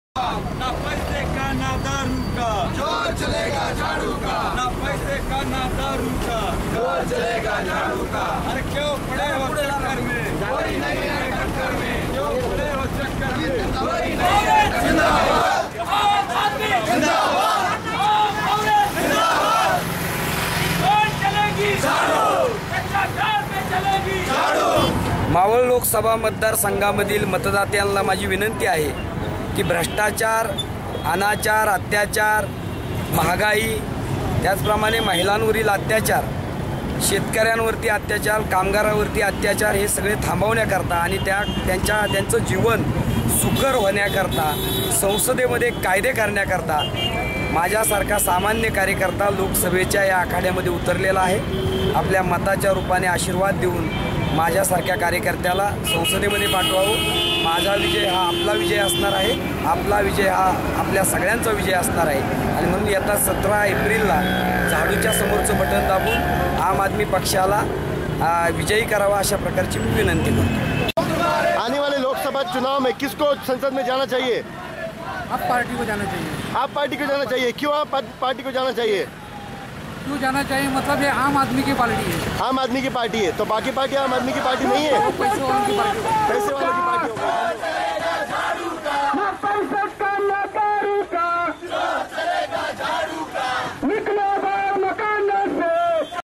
मारुती भापकर Maruti Bhapkar, Maval, Maharashtra (मराठी)
Aam Aadmi Party, Maval
Meet your Candidate for Maval constituency, Shri Maruti Bhapkar